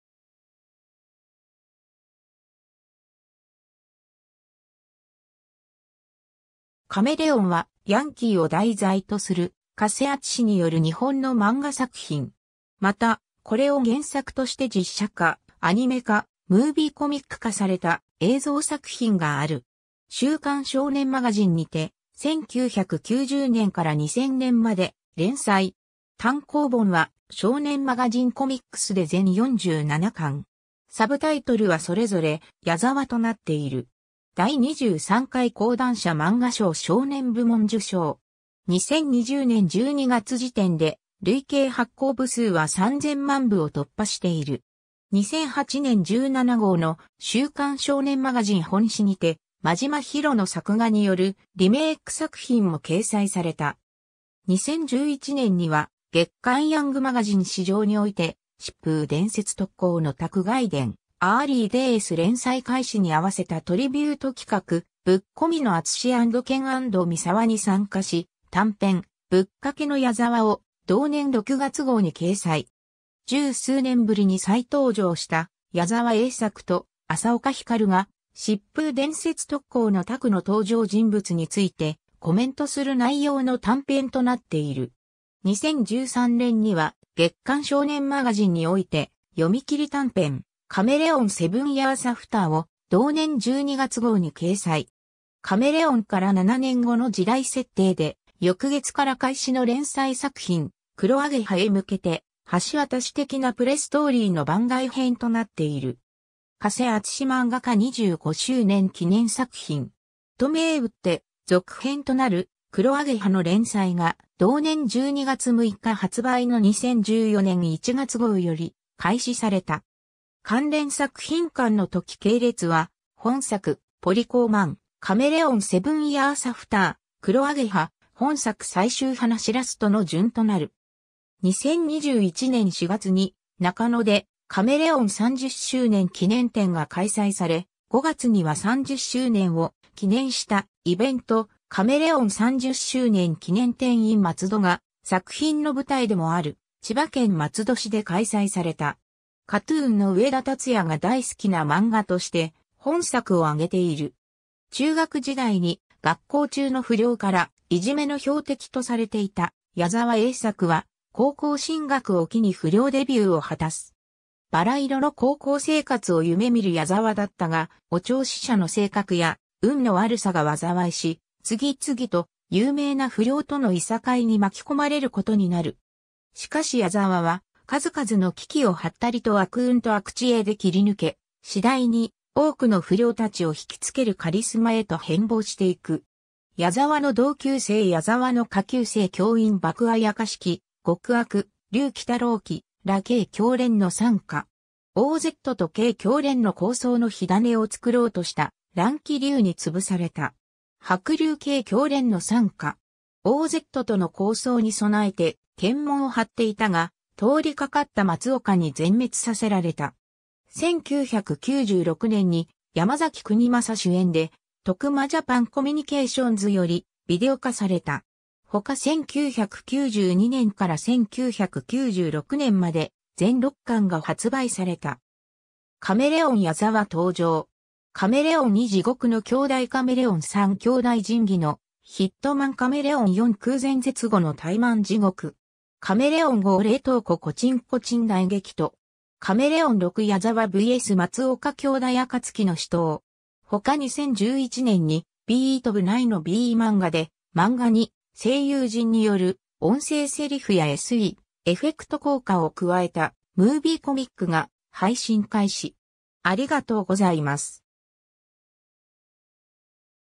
カメレオン (漫画)
不良少年・不良少女を主人公とした漫画作品
『カメレオン』は、ヤンキーを題材とする加瀬あつしによる日本の漫画作品。
また、これを原作として実写化（オリジナルビデオ）、アニメ化（OVA）、ムービーコミック化された映像作品がある。
 『週刊少年マガジン』（講談社）にて、1990年から2000年まで連載。
単行本は少年マガジンコミックスで全47巻。
サブタイトルはそれぞれ「YAZAWA･○○」となっている。
第23回講談社漫画賞少年部門受賞。
2020年12月時点で累計発行部数は3000万部を突破している。
 2008年17号の『週刊少年マガジン』本誌にて、真島ヒロの作画によるリメイク作品も掲載された。
 2011年には『月刊ヤングマガジン』誌上において、『疾風伝説 特攻の拓 外伝 〜Early Day’s〜』（原作：佐木飛朗斗、作画：所十三）連載開始にあわせたトリビュート企画「ぶっこみのあつし＆健＆ミサワ」（加瀬あつし・和久井健・地獄のミサワ）に参加し、短編「ぶっかけのヤザワ」を同年6月号に掲載。
十数年ぶりに再登場した矢沢栄作と浅岡ひかるが、『疾風伝説 特攻の拓』の登場人物についてコメントする内容の短編となっている。
 2013年には『月刊少年マガジン』において、読み切り短編『カメレオン SevenYearsAfter』を同年12月号に掲載。
『カメレオン』から7年後の時代設定で、翌月から開始の連載作品『くろアゲハ』へ向けて、橋渡し的なプレストーリーの番外編となっている。
「加瀬あつしマンガ家25周年記念作品!」と銘打って、続編となる『くろアゲハ』の連載が同年12月6日発売の2014年1月号より開始された。
関連作品間の時系列は、「本作」→「ポリ公マン」→「カメレオン　SevenYearsAfter」→「くろアゲハ」→「本作最終話ラスト」の順となる。
 2021年4月に中野で「カメレオン30周年記念展」が開催され、5月には30周年を記念したイベント「カメレオン30周年記念展 in 松戸」が作品の舞台でもある千葉県松戸市で開催された。
 KAT-TUNの上田竜也が大好きな漫画として本作を挙げている。
 中学時代に学校中の不良からイジメの標的とされていた矢沢栄作は、高校進学を機に不良デビューを果たす。
バラ色の高校生活を夢見る矢沢だったが、お調子者の性格や運の悪さが災いし、次々と有名な不良との諍いに巻き込まれることになる。
しかし矢沢は数々の危機をハッタリと悪運と悪知恵で切り抜け、次第に多くの不良達を惹きつけるカリスマへと変貌していく。
 ヤザワの同級生 ヤザワの下級生 教員 爆妖鬼 極悪龍 鬼太郎 鬼羅 京狂連の傘下。
OZと京狂連の抗争の火種を作ろうとした乱鬼龍に潰された。
 白龍 京狂連の傘下。
OZとの抗争に備えて検問を張っていたが、通りかかった松岡に全滅させられた。
 1996年に、山崎邦正主演で徳間ジャパンコミュニケーションズ（日本映像株式会社製作）よりビデオ化された。
 ほか 1992年から1996年まで、全6巻が発売された。
 カメレオン ヤザワ登場! カメレオン2 地獄のキョーダイ カメレオン3 兄弟仁義のヒットマン カメレオン4 空前絶後のタイマン地獄!! カメレオン5 冷凍庫コチンコチン大激斗! カメレオン6 ヤザワVS松岡兄弟 暁の死闘! ほか 2011年にBeeTV内のBeeマンガで、漫画に声優陣による音声セリフやSE、エフェクト効果を加えたムービーコミックが配信開始。